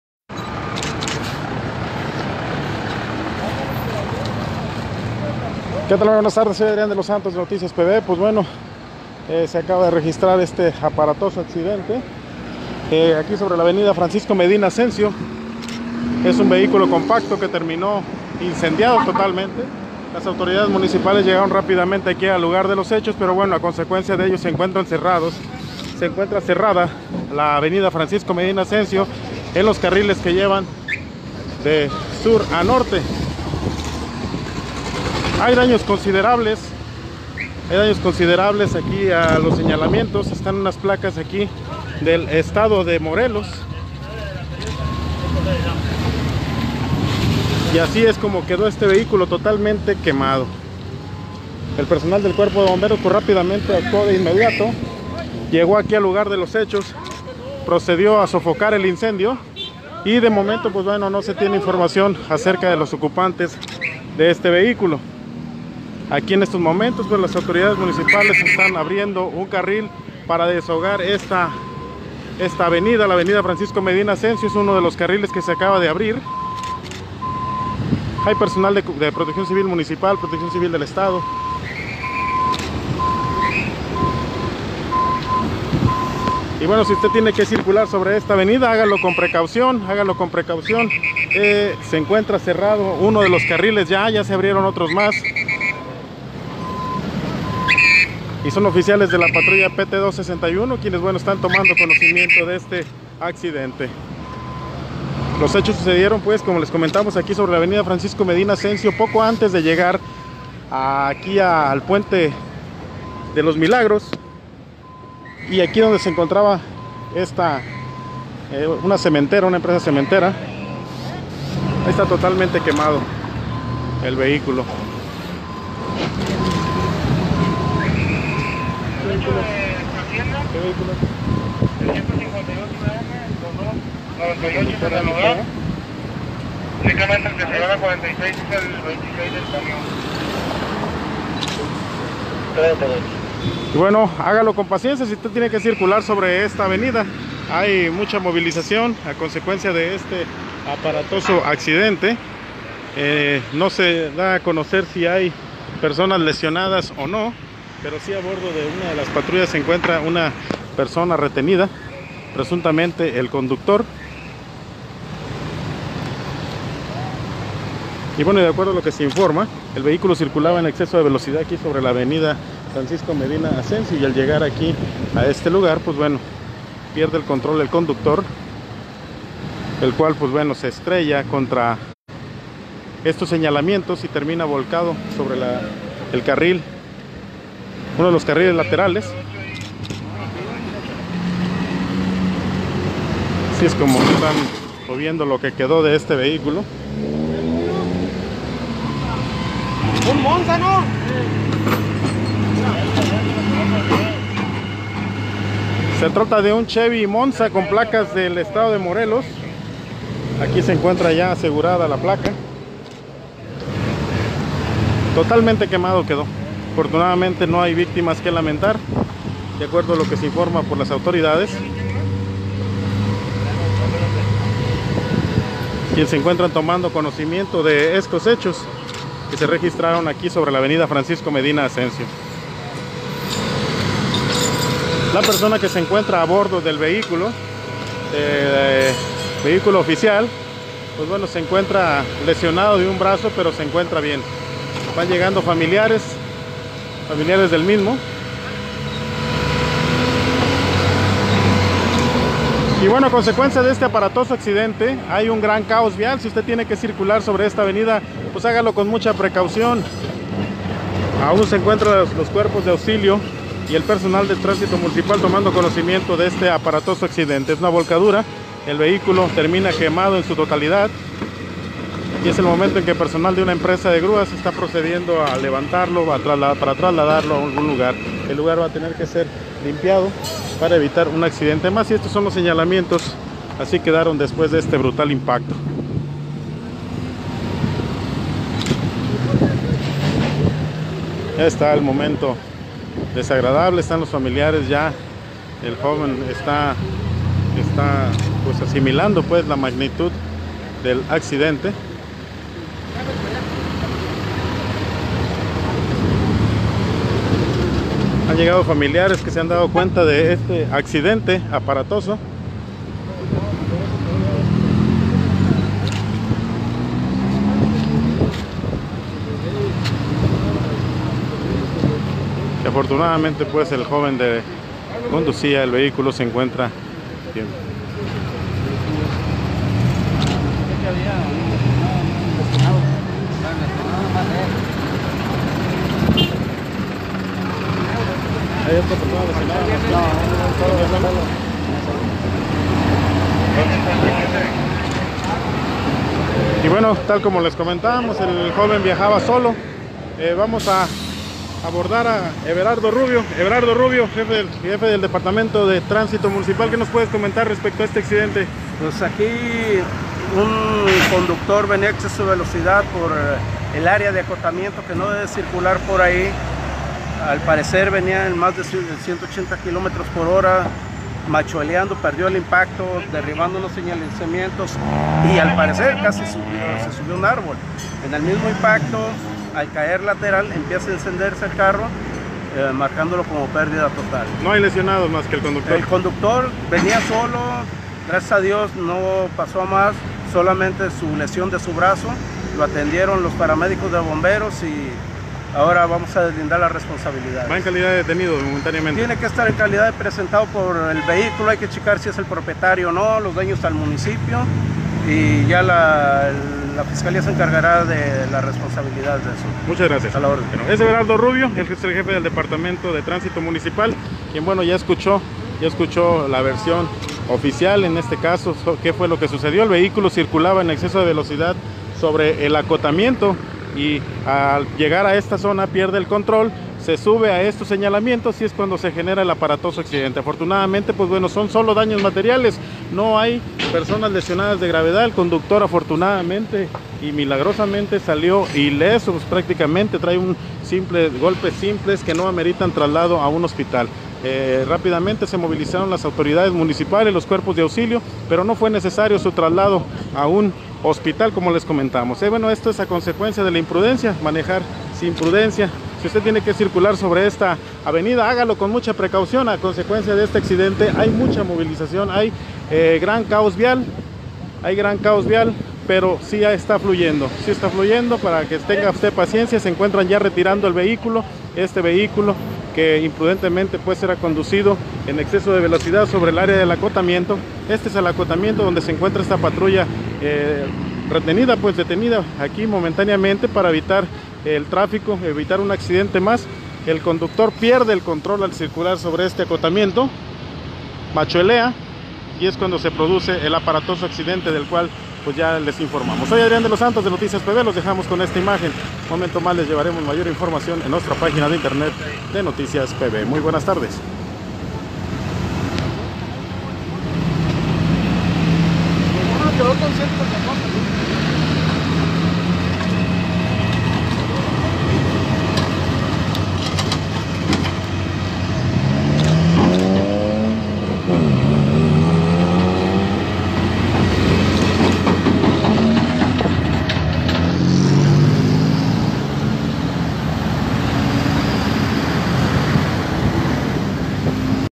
Aparatoso accidente sobre la avenida de ingreso, a la altura del Aeropuerto
Accidente | Iba a más de 180 km/h, el conductor perdió el control y terminó volcado, provocando con ello que la unidad se incendiara y terminara totalmente calcinada.

El conductor vive para contarlo. Es un joven que, por fortuna iba solo. Tras el accidente se generó un gran caos vial, que desquició el tráfico por varios minutos.